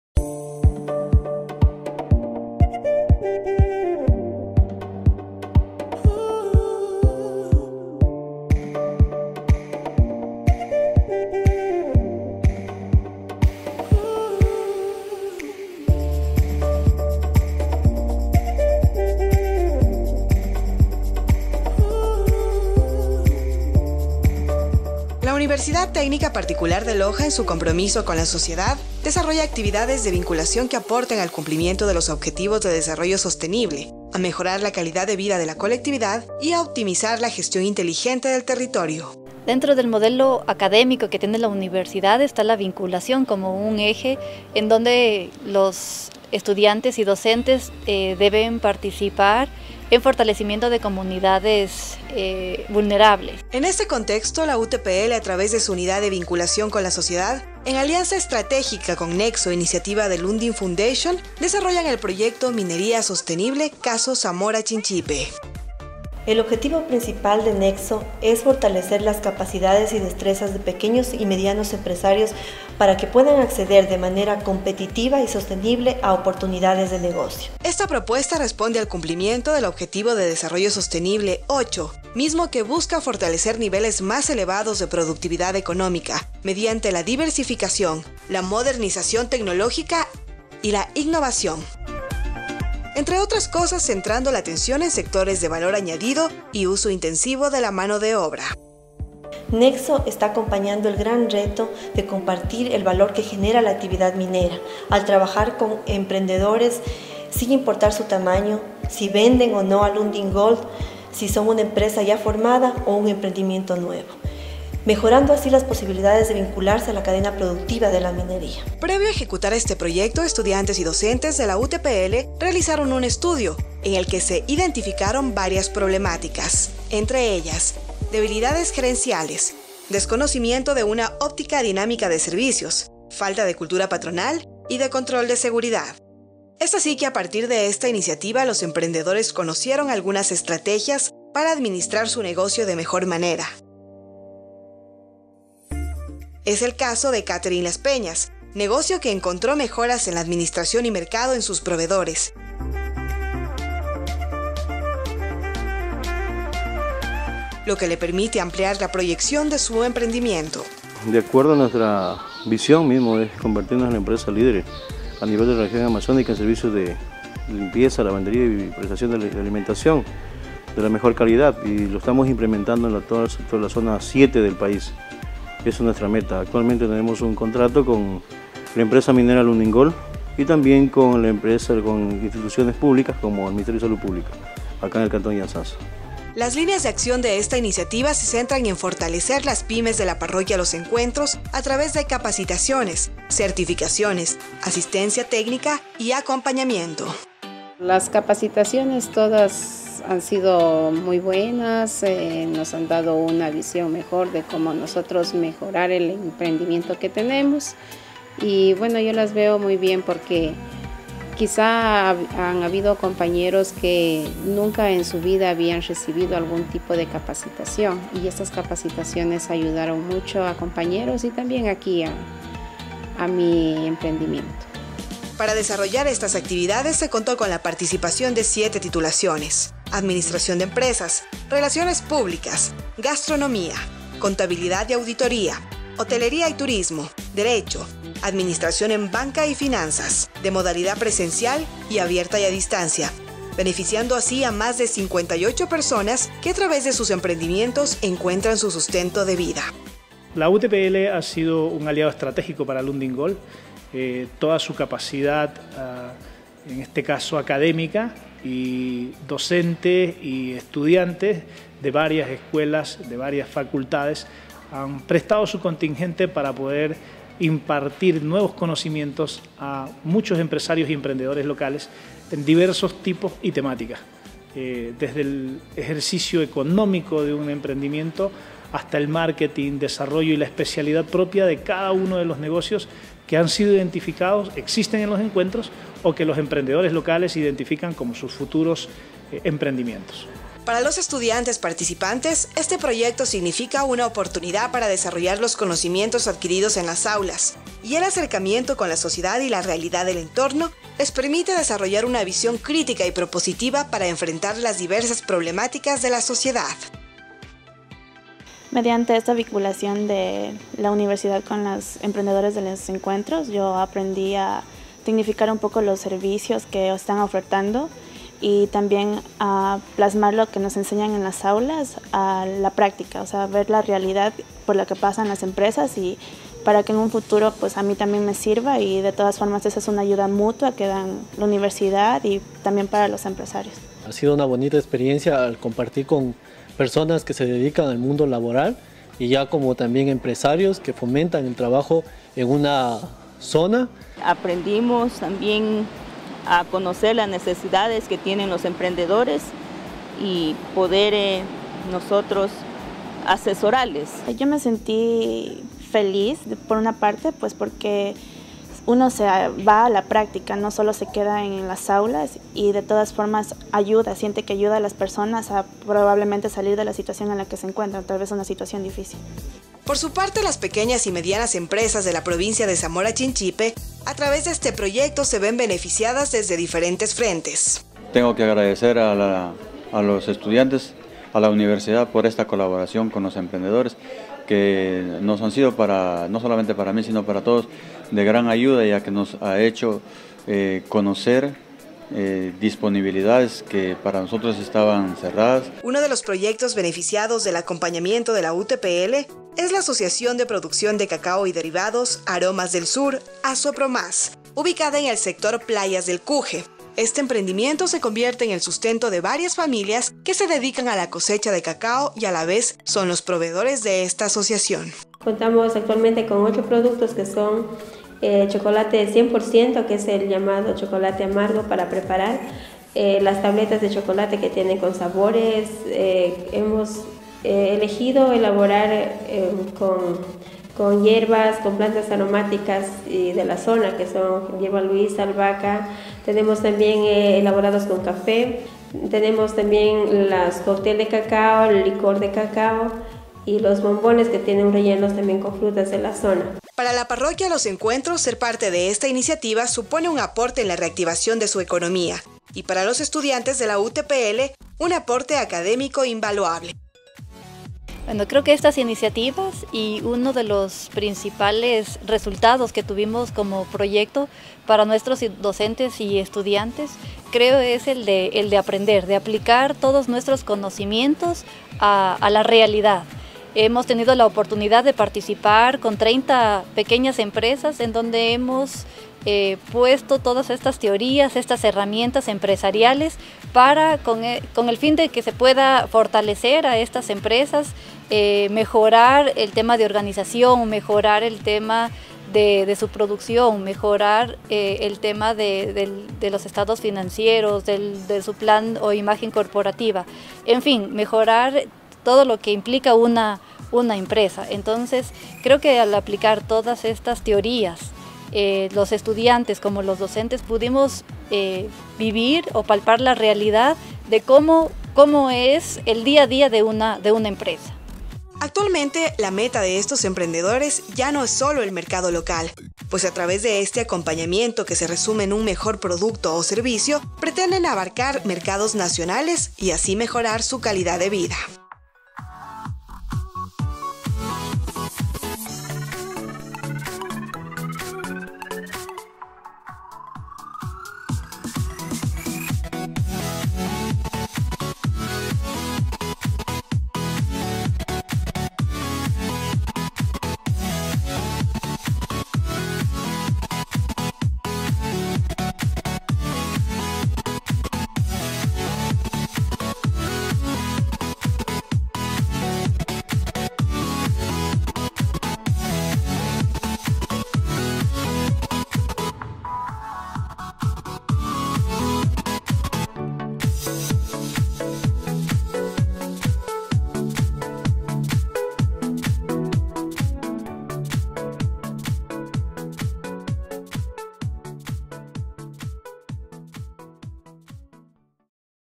Proyecto de Vinculación: Minería Sostenible caso Zamora Chinchipe
La UTPL dentro de su plan académico esta la vinculación como un eje donde los estudiantes y docentes deben participar en fortalecimiento de comunidades vulnerables. Es así como la UTPL y NEXO iniciativa de Lundin Foundation desarrollan el proyecto Minería Sostenible caso Zamora Chinchipe.